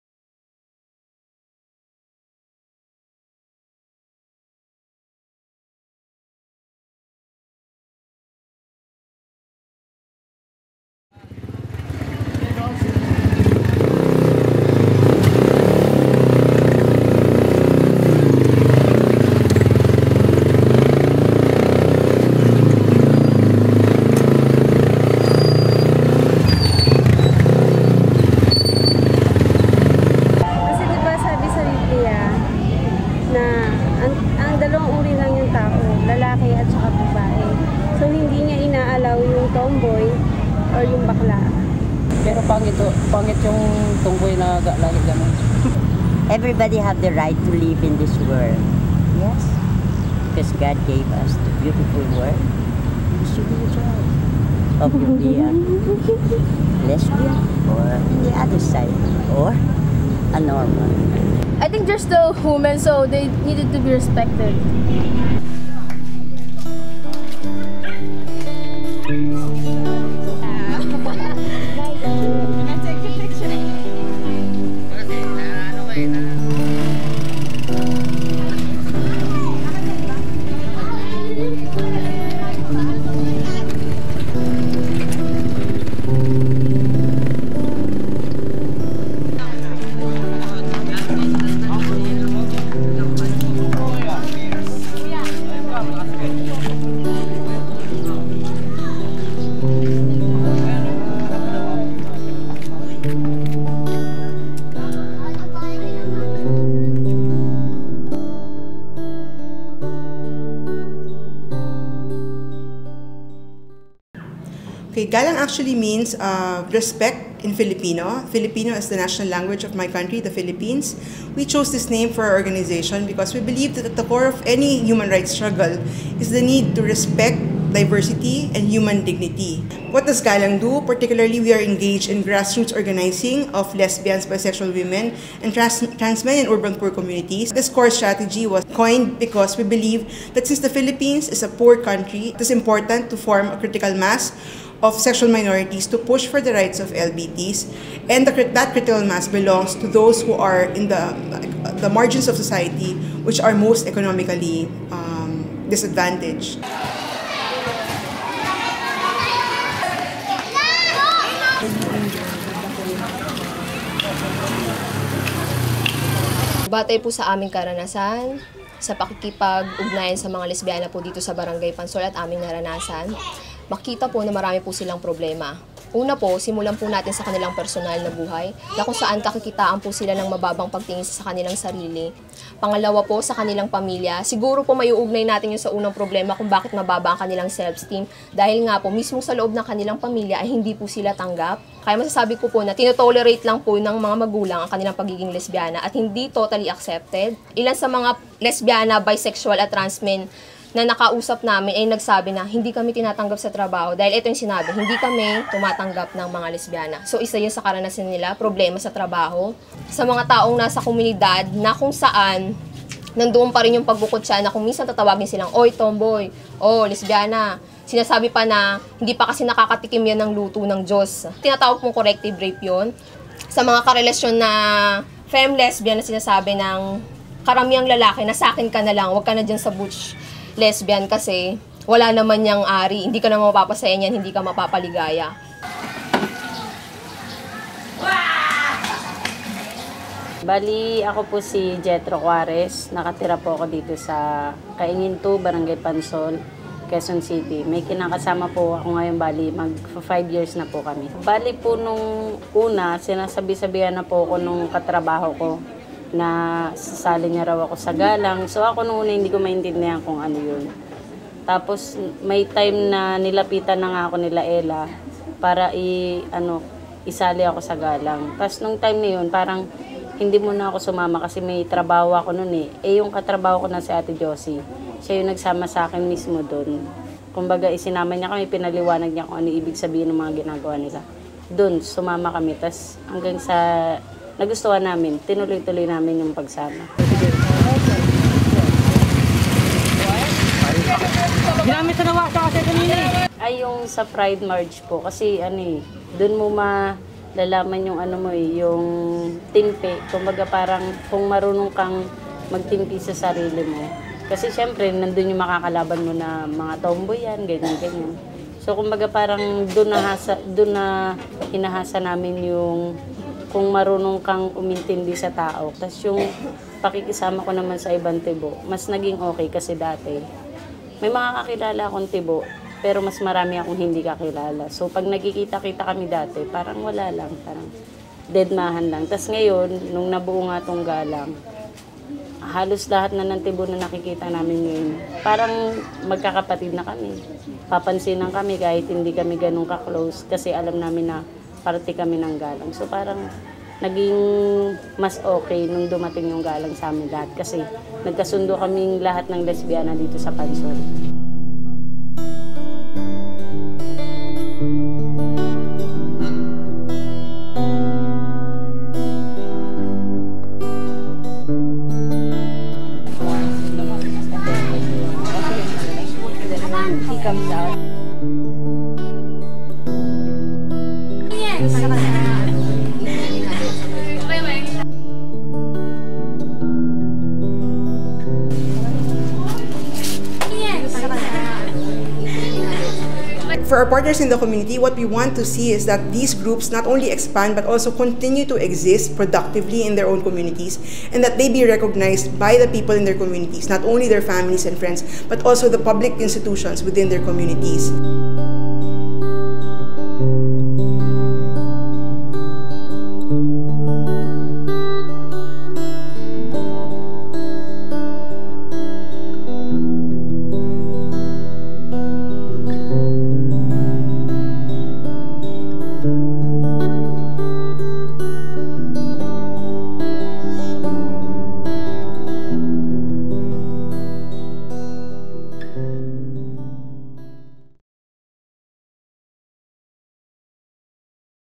Galang in the Grassroots
A short documentary video on Galang, the lesbian-run Philippine organization working with issues concerning lesbians, bisexual women and transmen in selected grassroots communities in Quezon City.

This video was produced on May 2011 and completed in June 2011 for Mama Cash Netherlands.

Direction/Script: Olivia Libay Linsangan Cantor
(Concept in consultation with Mama Cash)
Production team: Teta Tulay, Frances Mortel
Postproduction team: Kwami Biasbas, Dibayn Leano
Still photographer/production manager: Marnie Dolera